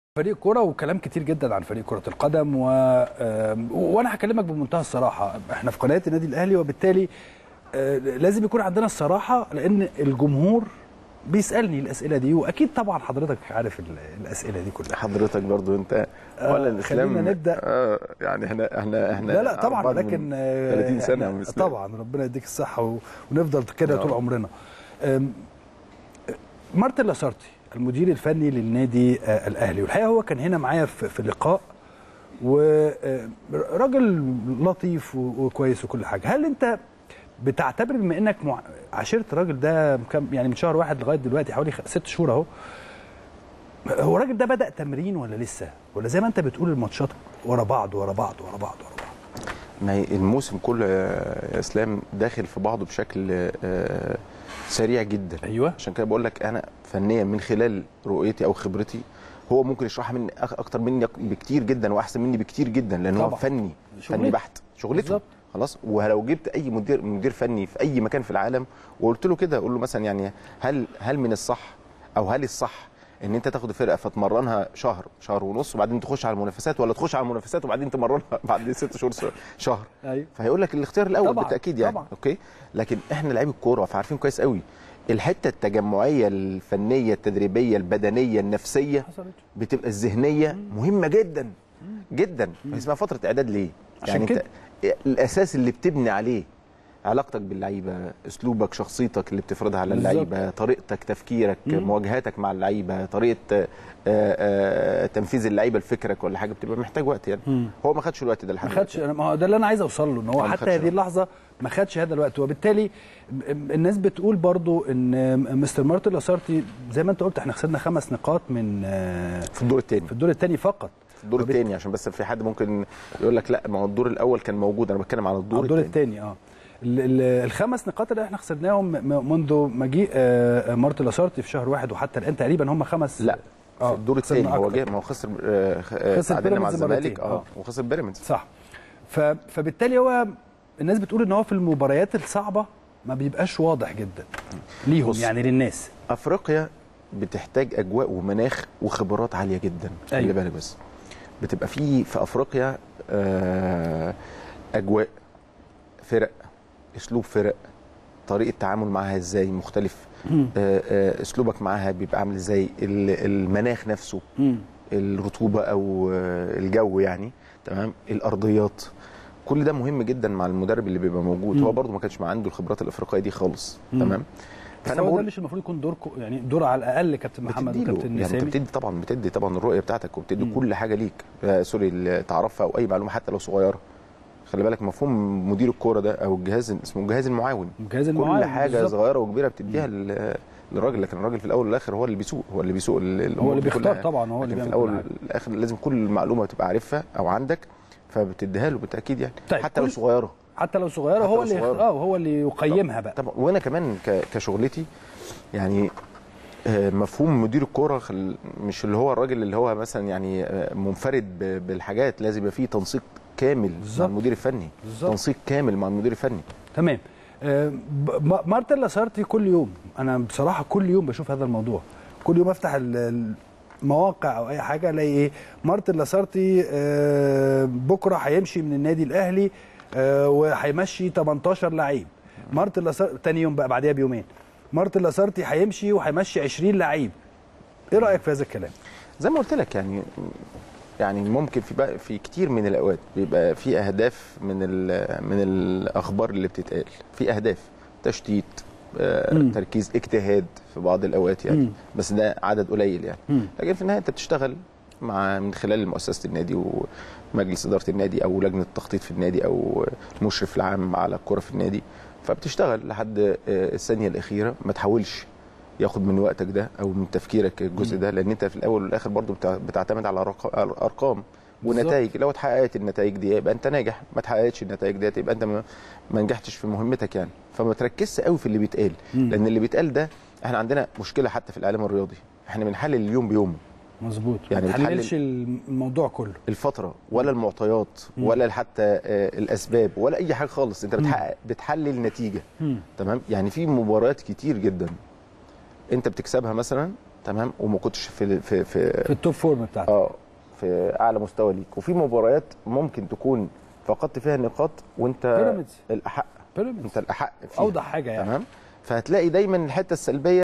سيد عبد الحفيظ : هناك اهداف من وراء اخبار رحيل "لاسارتى" الذى لم يحصل على فرصته الكاملة
التردد: 12341 أفقي.
القمر: نايل سات.
معامل الخطأ: 5/6.
معدل الترميز: 27500